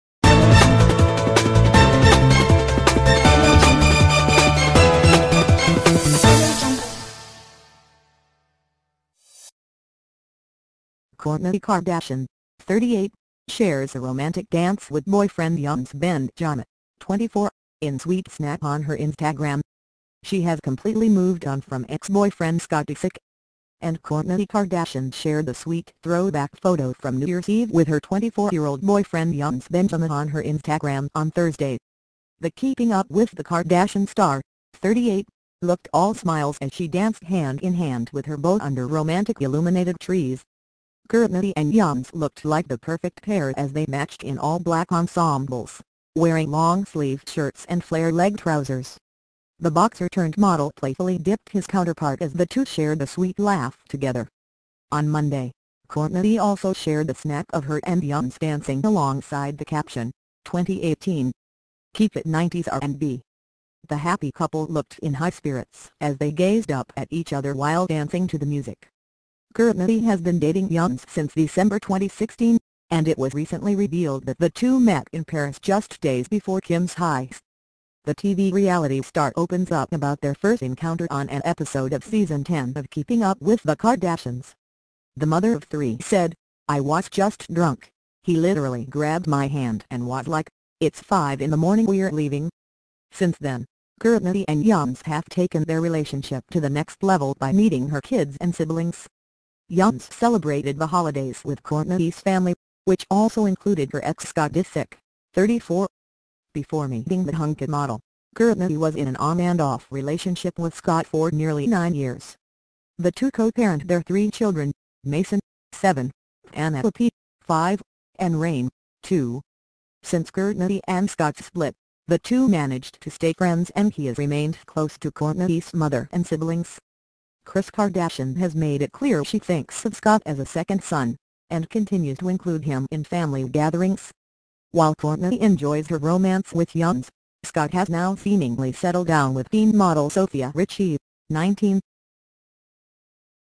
Kourtney Kardashian shares a romantic dance with boyfriend Younes Bendjima in on her Instagram
She has completely moved on from ex-boyfriend Scott Disick.
And Kourtney Kardashian shared a sweet throwback photo from New Year's Eve with her 24-year-old boyfriend Younes Benjima on her Instagram on Thursday.